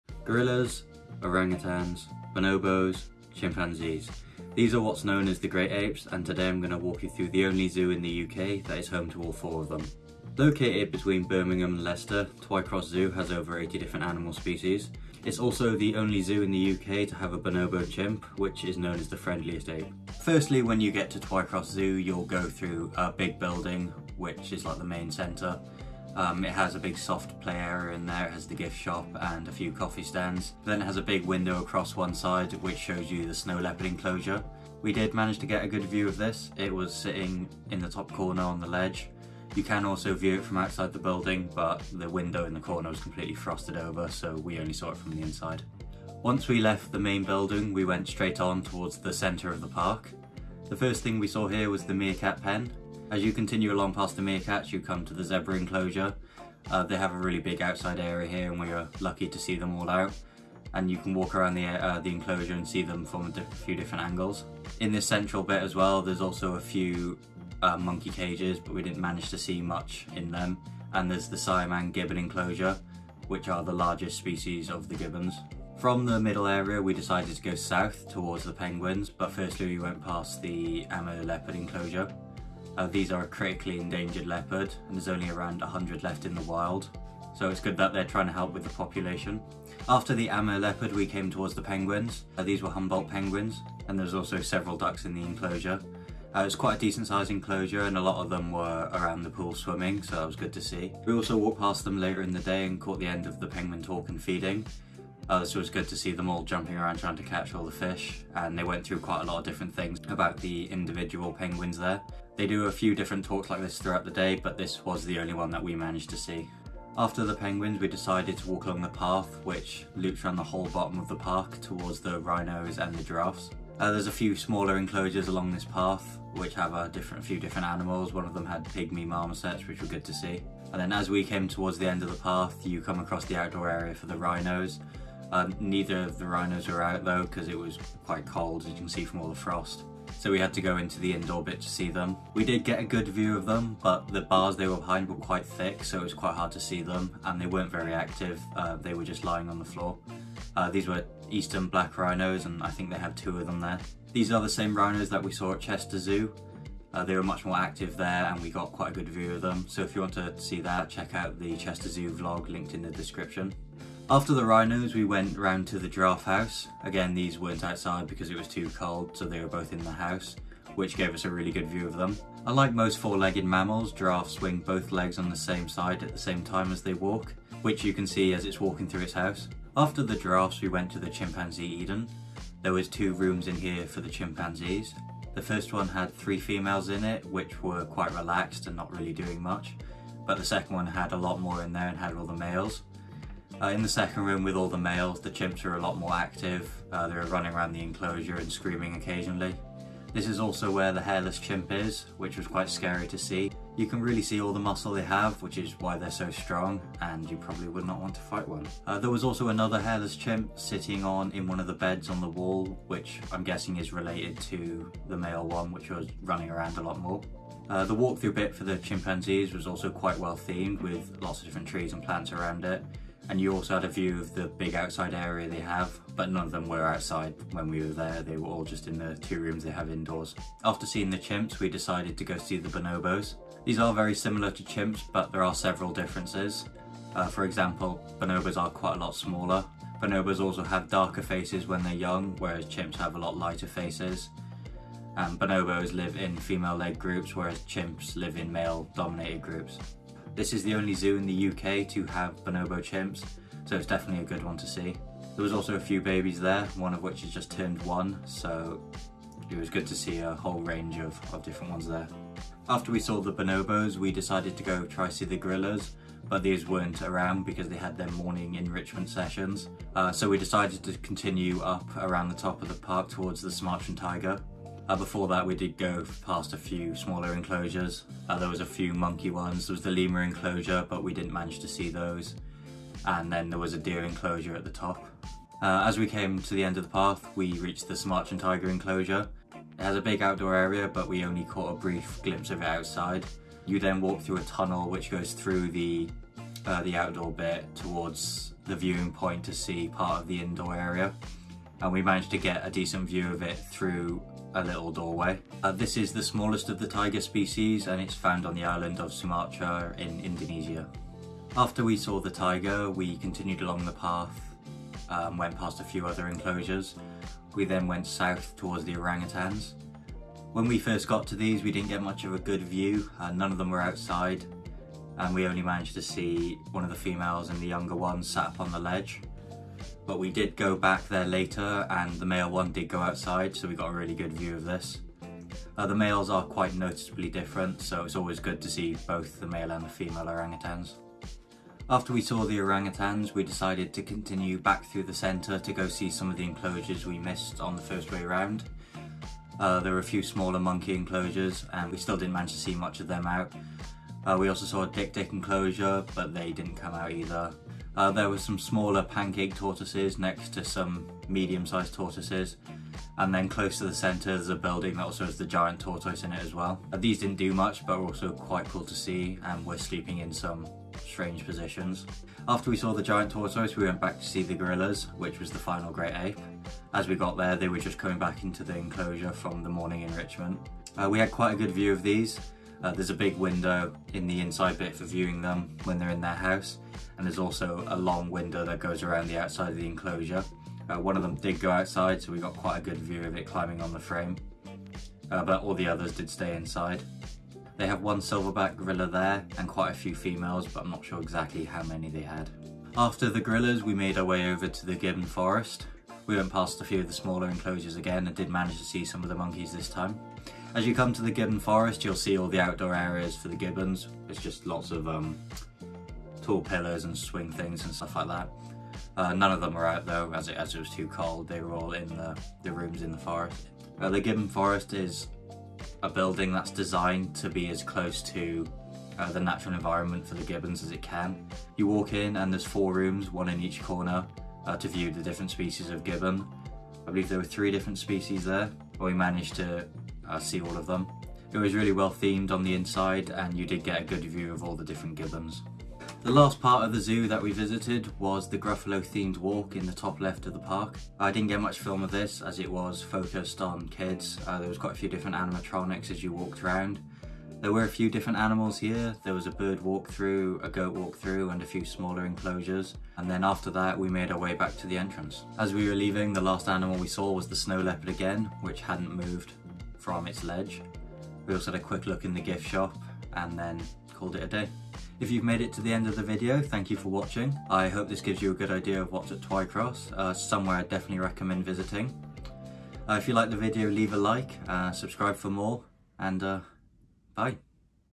This Zoo has all FOUR Great Apes! (Twycross Zoo Jan 2023)
In this video I take you around Twycross Zoo, the only zoo in the UK that's home to all four of the great apes, Gorillas, Orangutans, Bonobos, and Chimpanzees. There's also some rare animals such as the critically endangered Amur Leopard and many more!

If you want to find out more about Twycross Zoo, check out their

My Guitar Strap Brand: